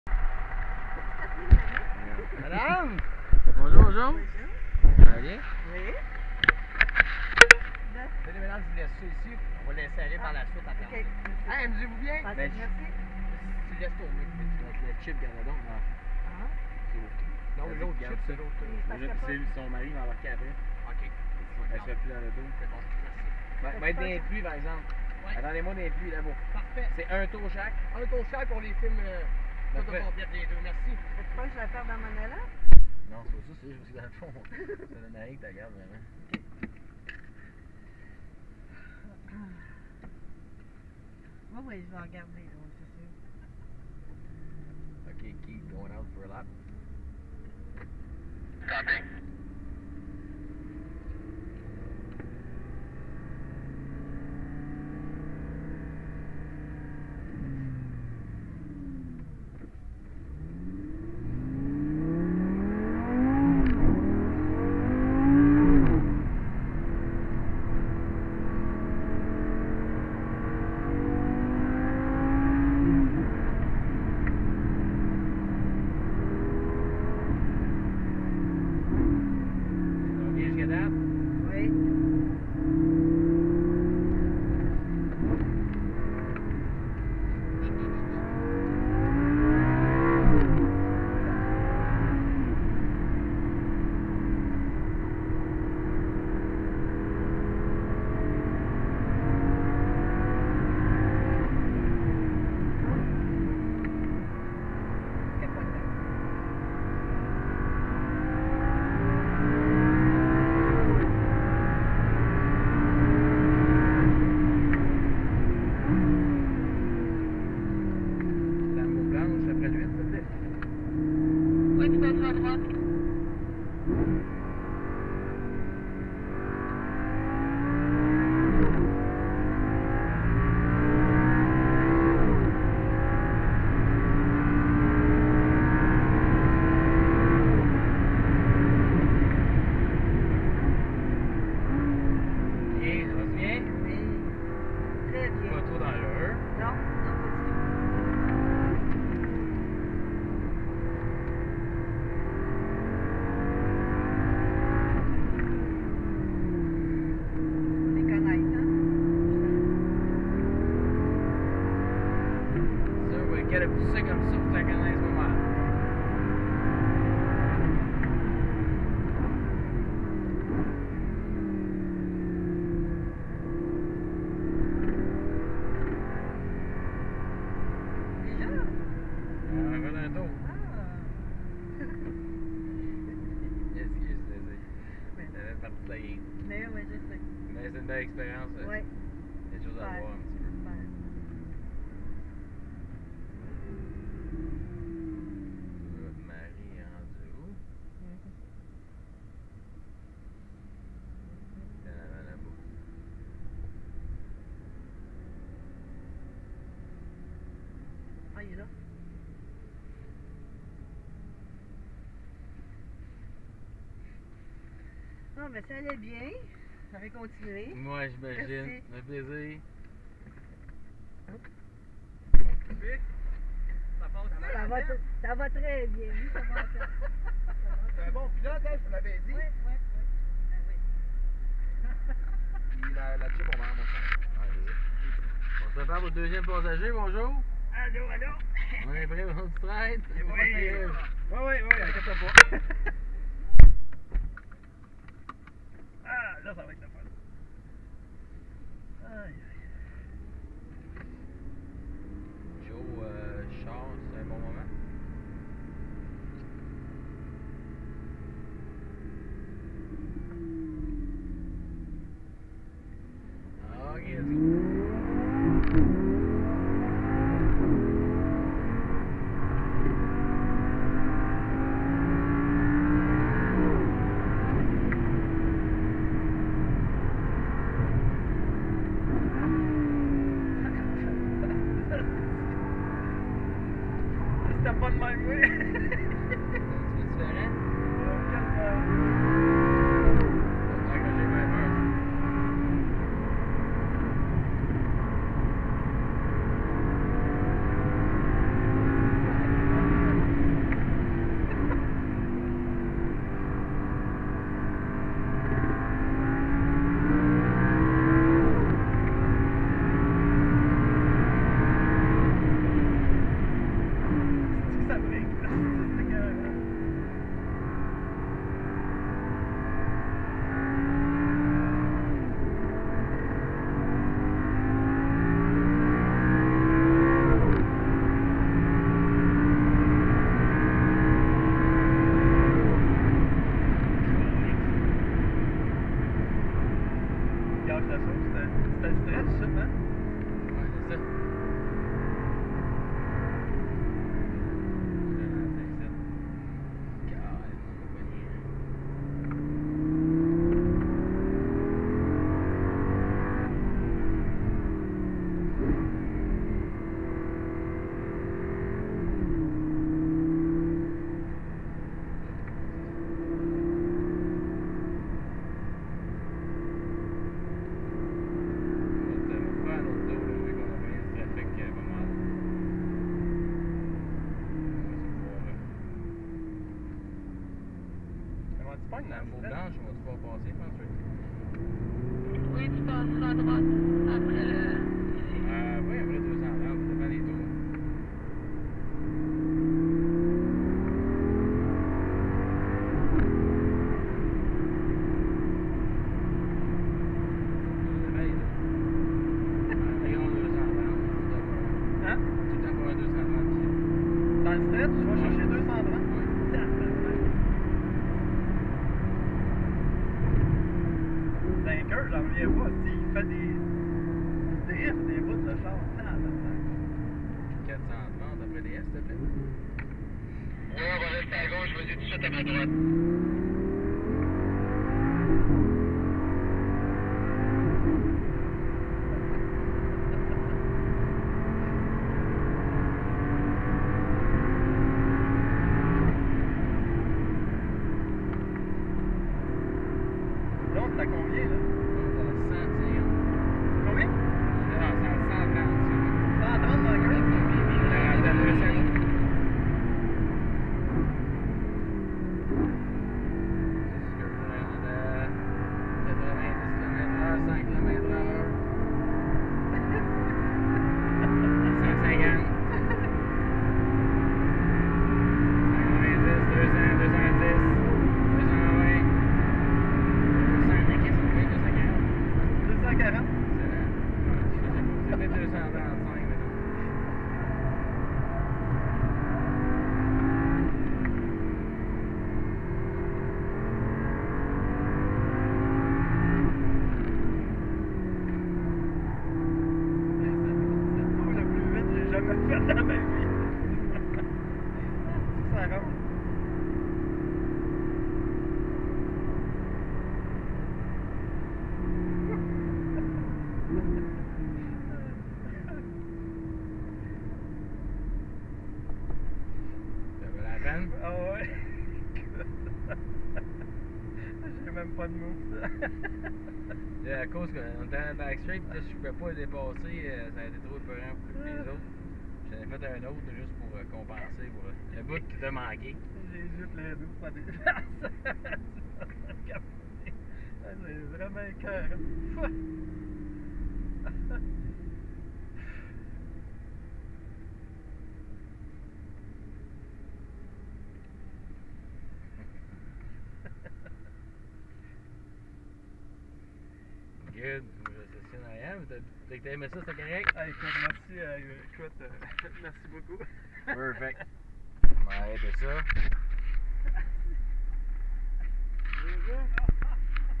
Manon & Daniel avec Jean François Dumoulin circuit Mont Tremblant
Manon et moi-même avec Jean-François Dumoulin au volant de la Lamborghini. Moi j'ai eu droit à 2 tours car ont a été ralentie par d'autres personnes. Ça vaut vraiment la peine de faire un tour supplémentaire avec un pilote professionnel comme Jean-François pour vraiment apprécier toutes la puissance de ce bolide, car je peux vous dire qu'il pousse vraiment à fond. Différence avec moi, il a atteint 240 km/hres. et il entrait dans des courbes asser vite merçi...Oh yeah !
Un grand merçi à Jean-François Dumoulin, très bon pilote.